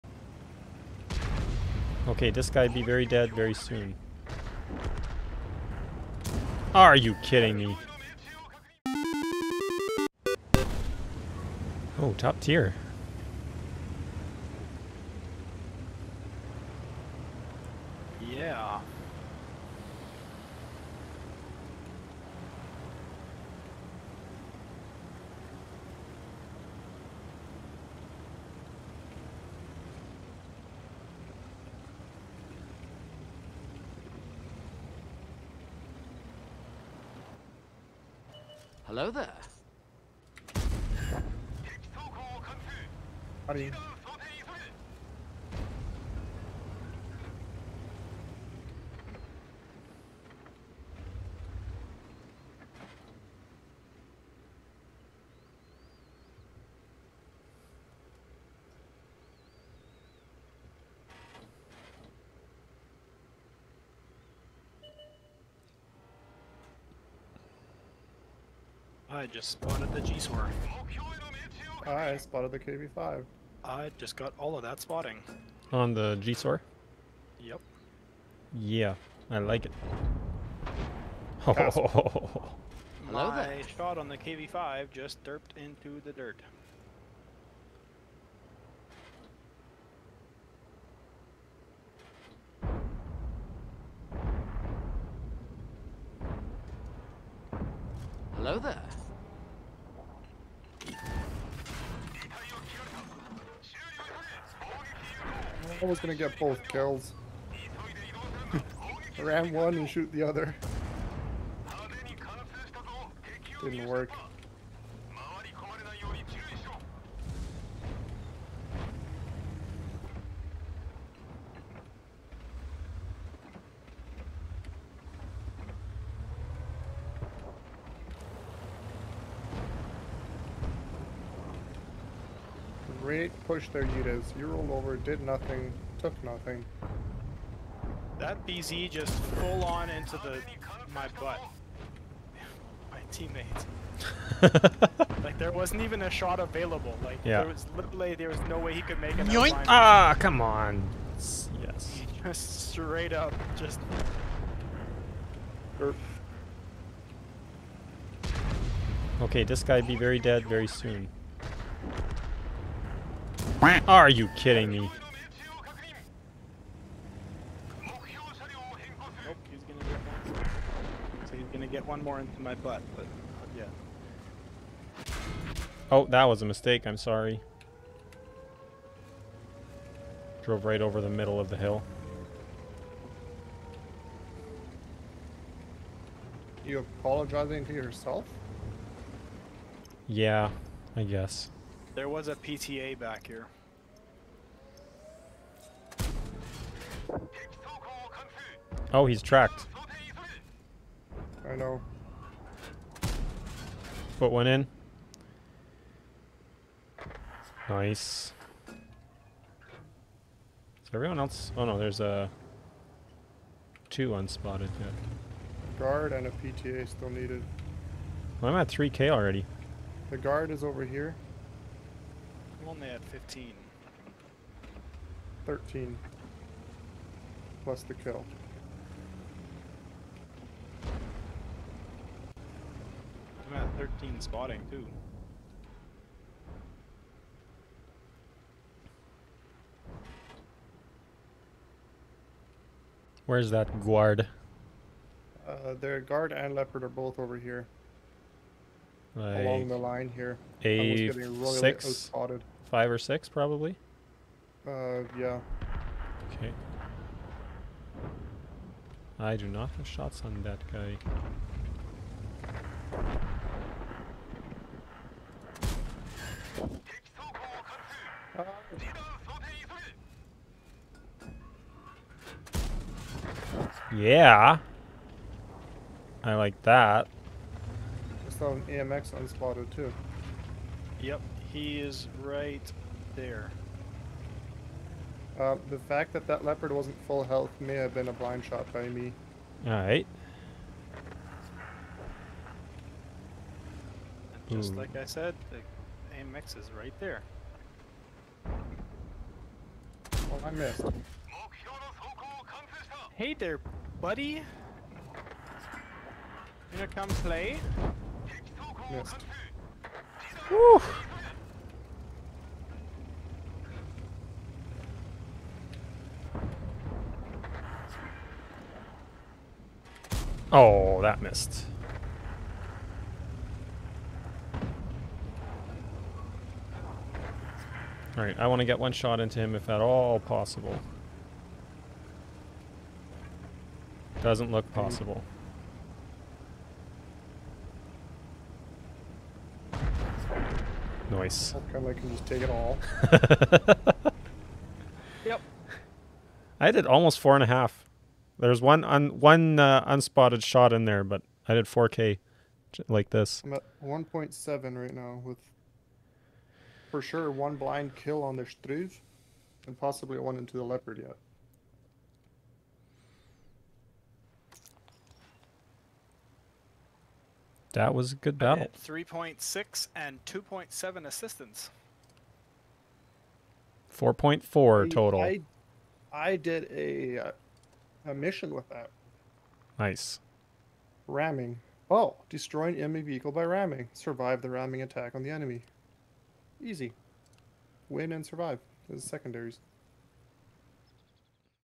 Type 61 4.4k Damage Triple Platoon
This was a solid battle right on the tail end of my time with the Type 61. Shortly after this I researched and bought the STB-1!